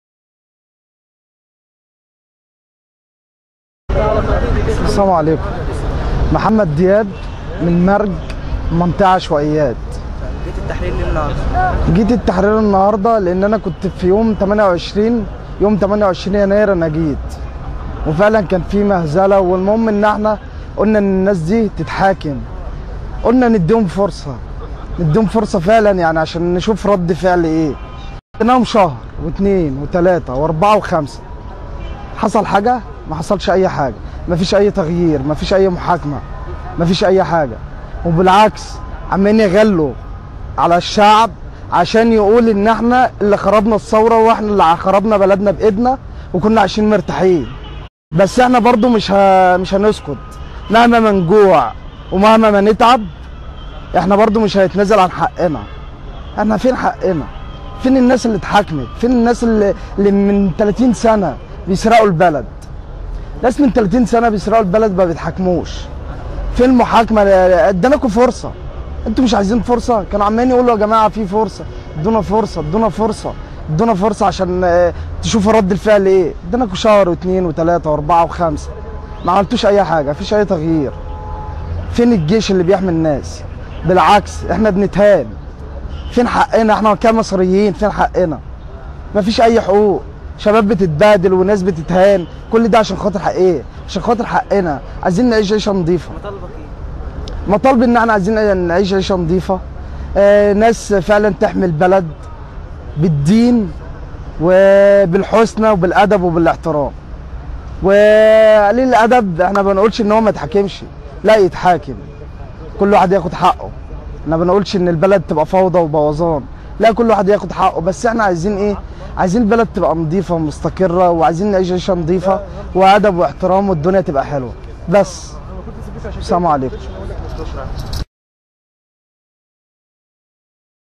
مؤامرات من النظام الحاضر على الشعب من ميدان التحرير
مواطن يقول تعليقه فى  مؤامرات من النظام الحاضر على الشعب من ميدان التحرير